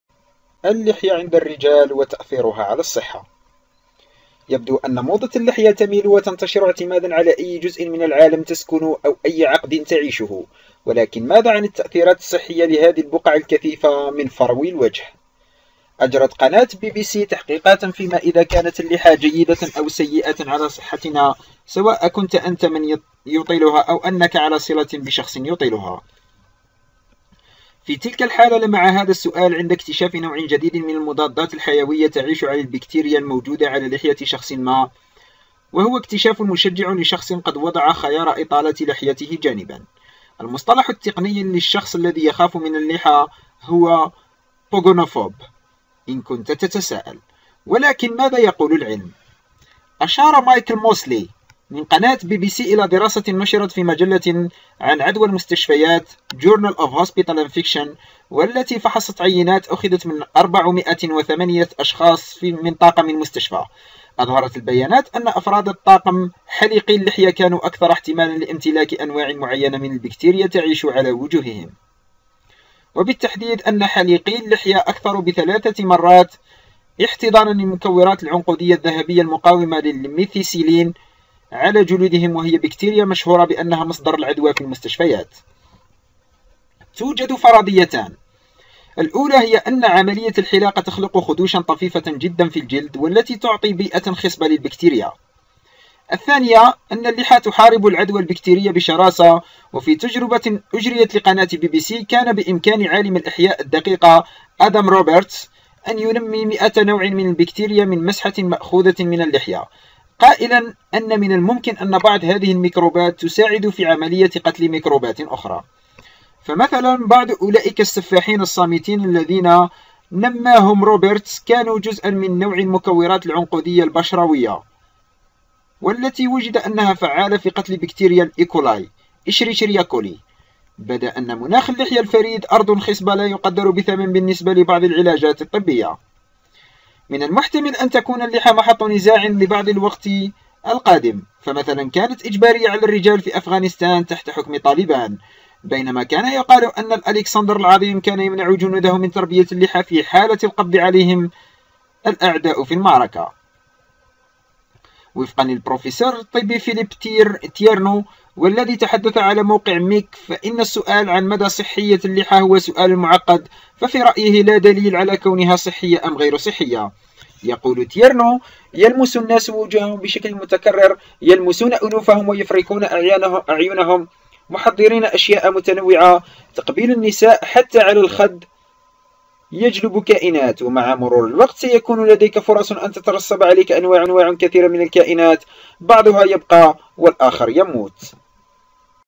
اللحية عند الرجال وتأثيرها على الصحة
يبدو أن موضة اللحية تميل وتنتشر اعتمادًا على أي جزء من العالم تسكن وأي عقد تعيشه، ولكن ماذا عن التأثيرات الصحية لهذه البقع الكثيفة من (فرو الوجه)؟